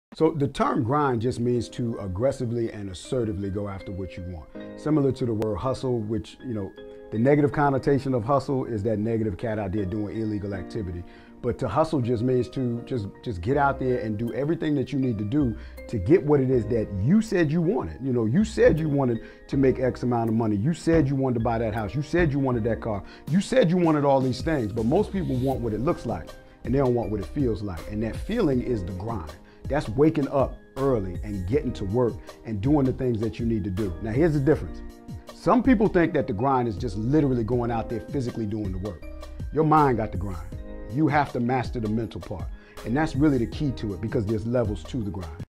MASTER THE MENTAL PART: Kendall Ficklin
Master the mental part. You can't let your mood affect your moves!

Kendall is a "full time" entrepreneur for 20 years. He is committed to helping others become successful in their own lives through personal development sessions, corporate training, professional development workshops and student empowerment sessions.